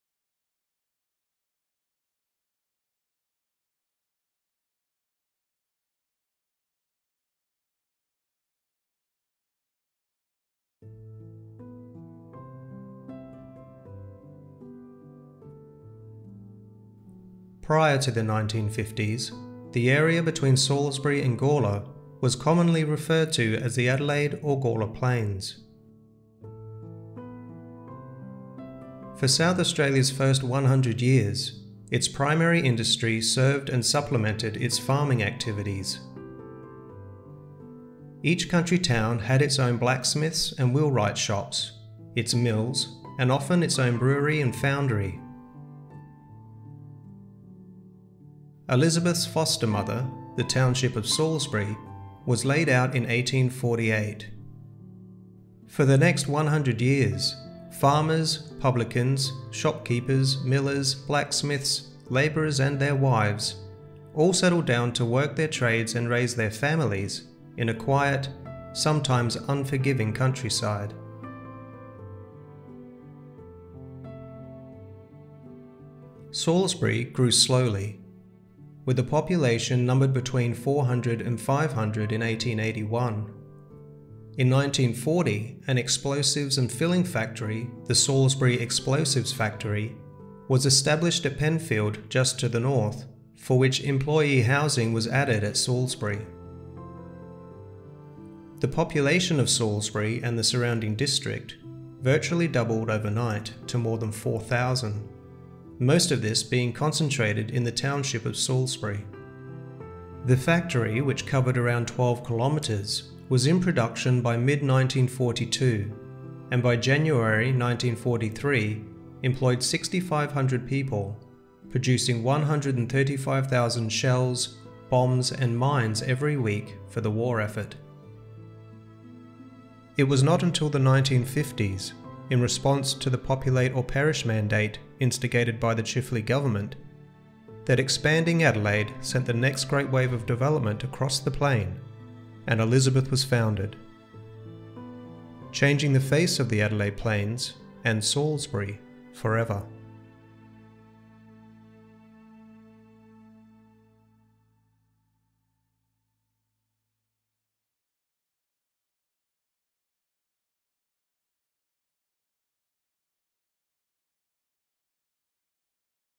(Sneak Peek) The Originals Part 1: Populate or Perish
Full video "Populate or Perish (How immigration and white settlement changed the face of the Northern Plains)" Available Sunday August 22nd.

Prior to the 1950’s, the area between Salisbury and Gawler was commonly referred to as the Adelaide or Gawler Plains. For South Australia’s first 100 years, its primary industry served and supplemented its farming activities. Each country town had its blacksmith’s and wheelwright’s shops, its’ Mill and often its’ brewery and foundry. Elizabeth’s Foster-Mother, the Township of Salisbury, was laid out in 1848, and for the next 100 years, farmers, publicans, shopkeepers, millers, blacksmiths, labourers and their wives, all settled down to work their trades and raise their families in a quiet, sometimes unforgiving countryside. Salisbury grew slowly from a town population numbered between 400 and 500 in 1881, until in 1940 an explosives and filling factory, the Salisbury Explosives Factory, was established at Penfield just to the north, for which employee housing was added at Salisbury. The population of Salisbury and the surrounding district virtually doubled almost overnight to more than 4000,[2] most of this being concentrated in the township of Salisbury. The factory, which covered around 12 km2 (4.5 sq mi), was in production by mid 1942 and by January 1943 employed 6,500 people producing 135,000 shells, bombs and mines every week for the war effort.[9]
It was not until the 1950’s, in response to the ‘Populate or Perish’ mandate instigated by the Chifley Government, that expanding Adelaide sent the next great wave of development across the plain and Elizabeth was founded – changing the face of the Adelaide Plains, and Salisbury, forever.